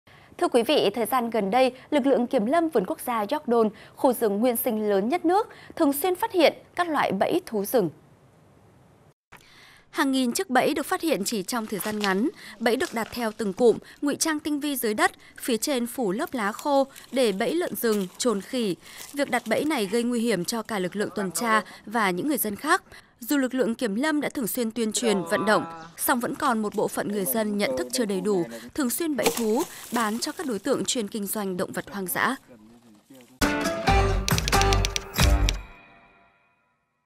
NGUY HIỂM TỪ BẪY THÚ RỪNG | VTV5
Hàng nghìn chiếc bẫy được phát hiện chỉ trong thời gian ngắn. Bẫy được đặt theo từng cụm, ngụy trang tinh vi dưới đất, phía trên phủ lớp lá khô để bẫy lợn rừng, chồn, khỉ.. Việc đặt bẫy này gây nguy hiểm cho cả lực lượng tuần tra, và những người dân khác.